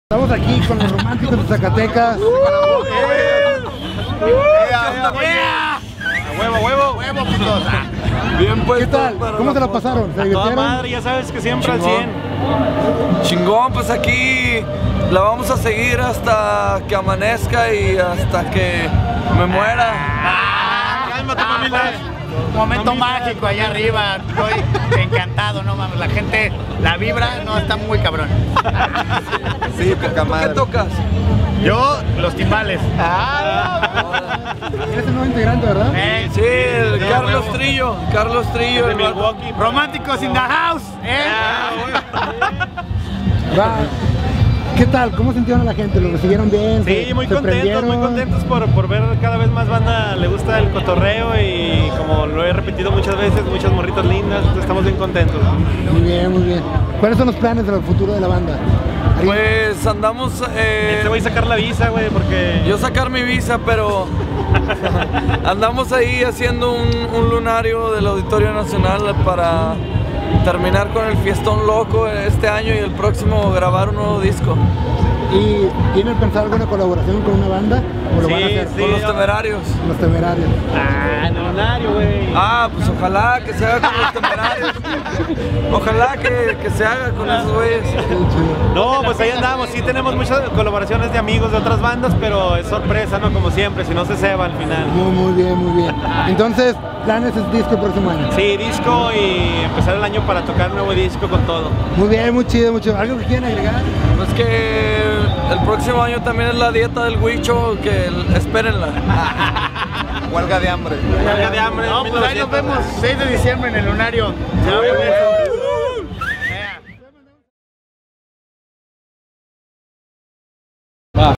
Entrevista a "Los Románticos de Zacatecas" by Jester TV
Nos encontramos a "Los Románticos de Zacatecas" en el "Pa´l Norte Rock Festival 2013" y como siempre se portaron de poca madre. Pudimos platicar con ellos, bromear y pasar un buen rato. Esperemos que disfruten la entrevista.